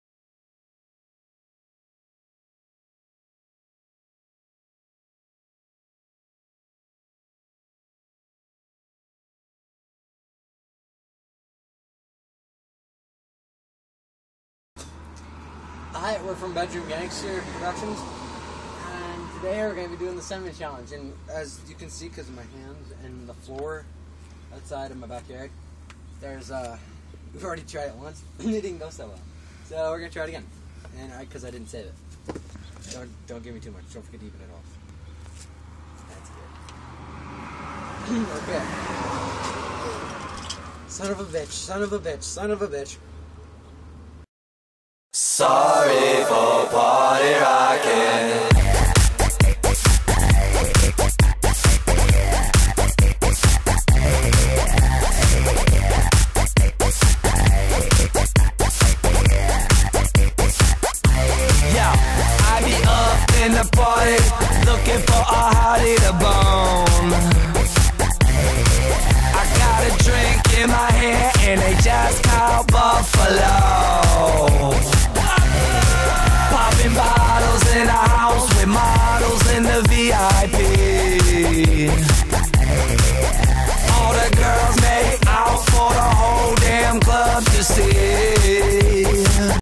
cinnamon challange.
cinnamon challange, with connor, jesse ,and trystan